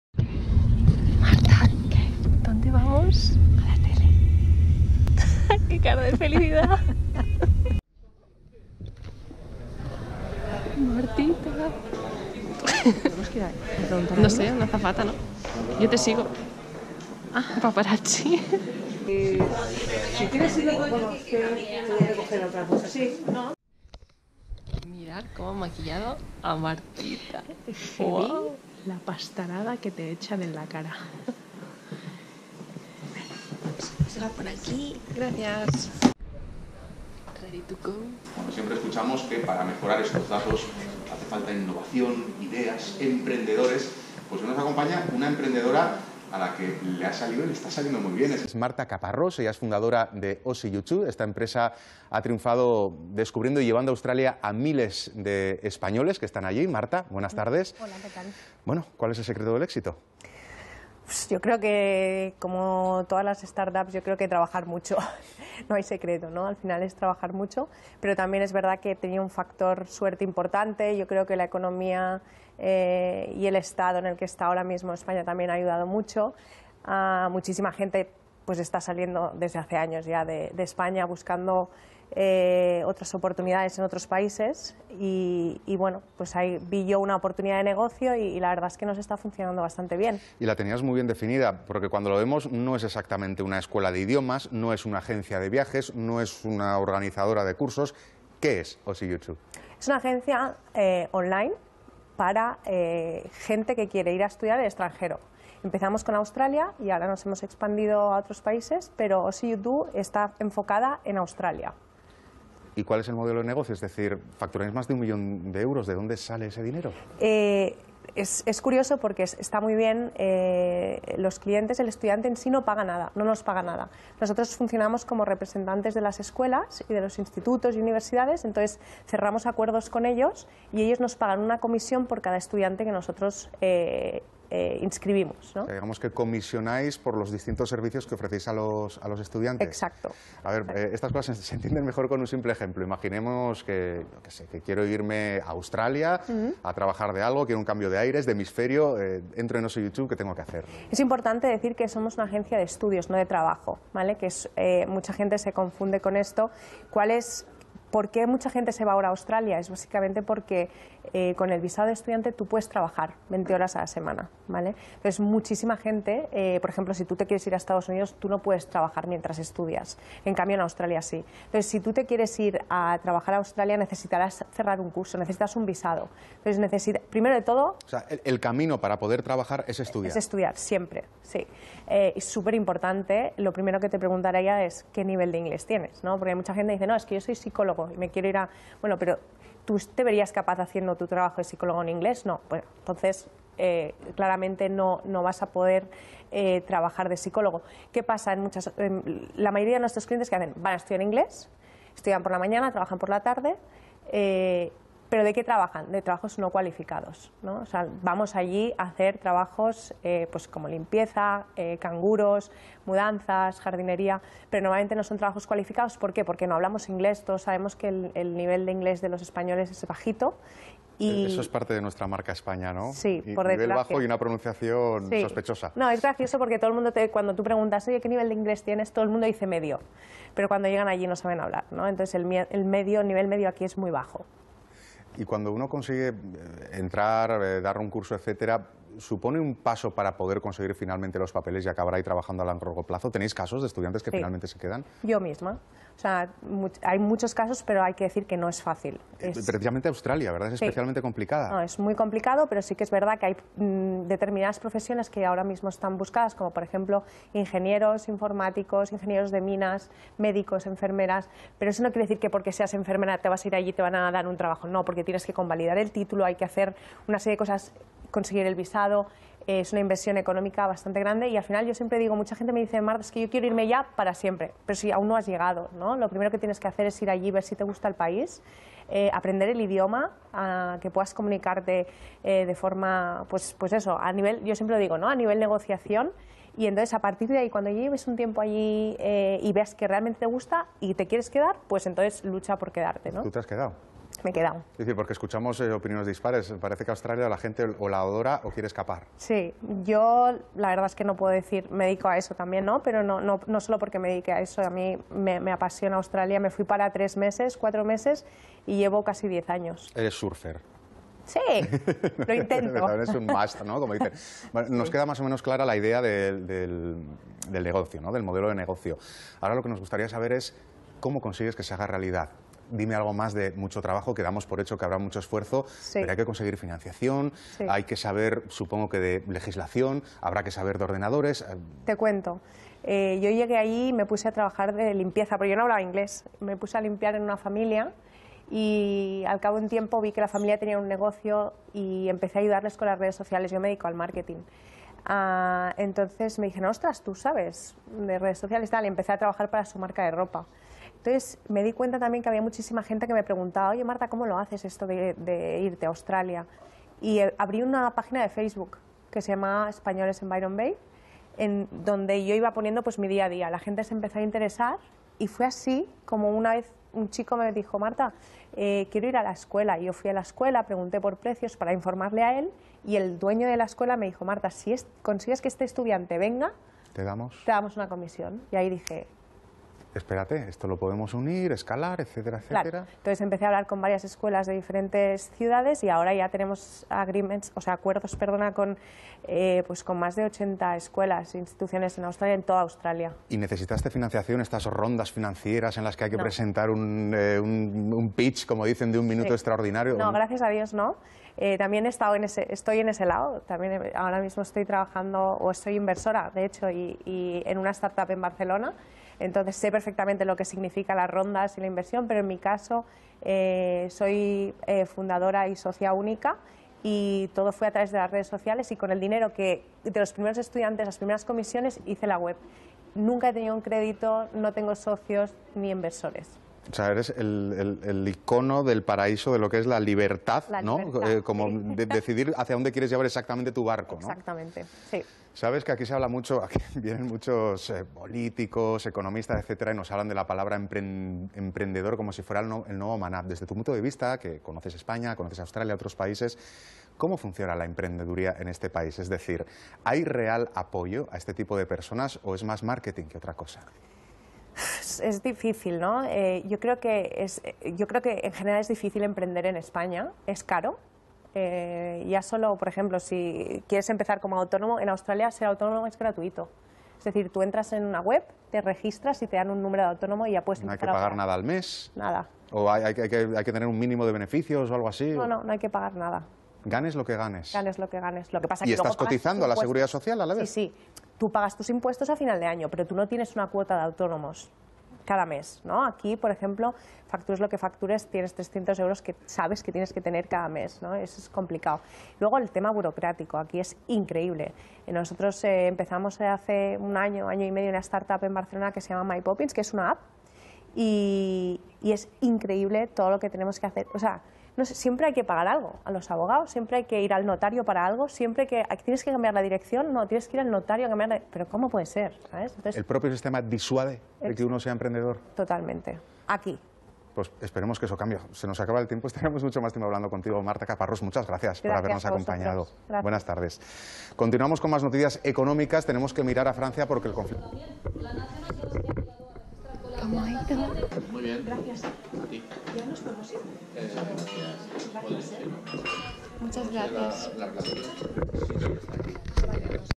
Nos entrevistan en RTVE - Making Of
El lunes 3 de diciembre vivimos una experiencia diferente. ¡Nos entrevistaron en directo en la sección Emprende del canal de Televisión 24h de RTVE!

Marta y Maria fueron a Madrid para asistir a la entrevista. ¡Qué emoción! Les encantó la experiencia.

En este vídeo podéis ver la llegada al plató de 24h de RTVE, maquillaje, entrevista en directo detrás de las cámaras y en pantalla y es postentrevista.

¡Muchas gracias por la oportunidad!